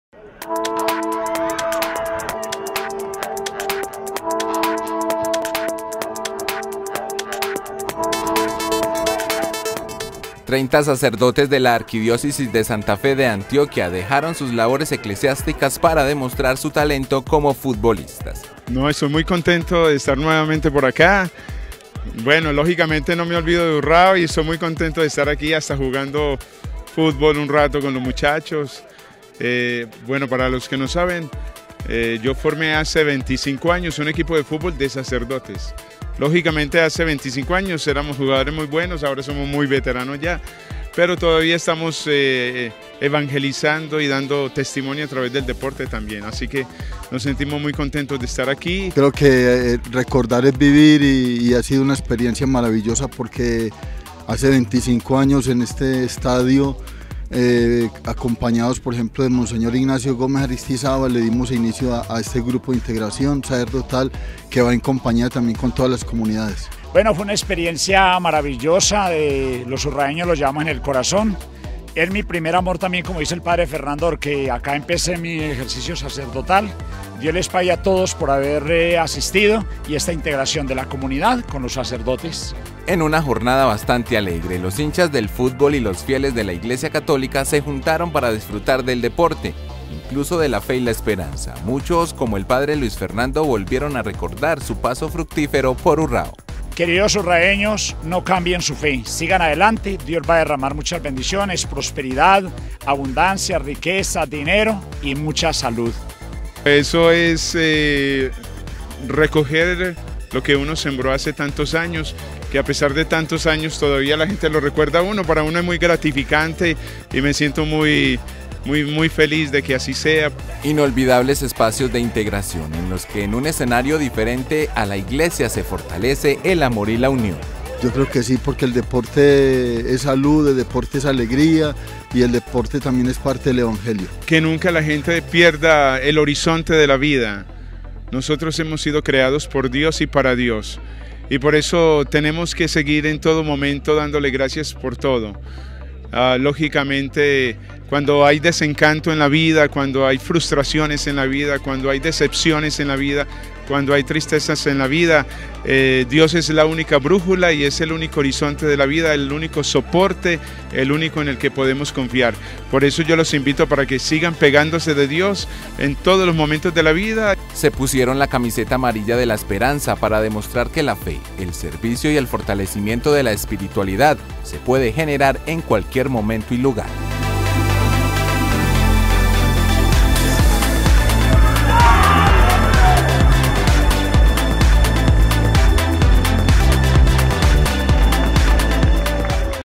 Sacerdotes y conductores pusieron a rodar el balón
Con la presencia del Padre Fernando Echeverry se llevó a cabo un encuentro deportivo entre sacerdotes de la Arquidiócesis de Santa Fe de Antioquia y la empresa de transporte Cootraur